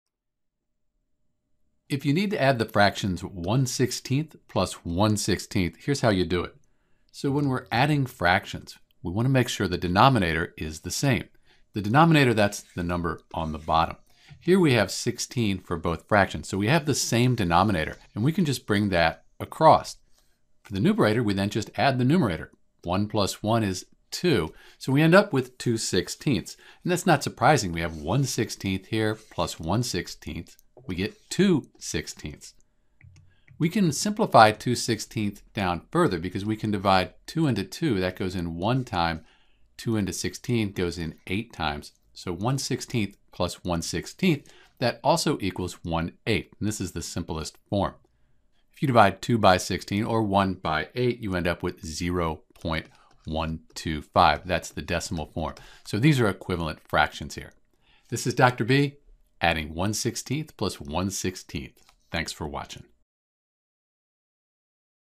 How to Add 1/2 and 1/16
Note that the lowest common multiple (LCM) is also called the lowest common denominator (LCD). You can find the LCM for 2 and 16 by listing all the multiples for 2 and 16. Then you select the lowest number that is the same for both numbers.

If we look at the multiples of 2 and 16, we'll find that both 2 and 16 can be divided evenly into 16. So 16 is the lowest common multiple. We multiply the 1/2 by 8/8to get 8/16.  We can now add 8/16 +to 1/16 to get 9/16.

A quick way to check your work is provided at the end of the video. When you add 1/2 + 1/16 you should get the same fraction for both methods.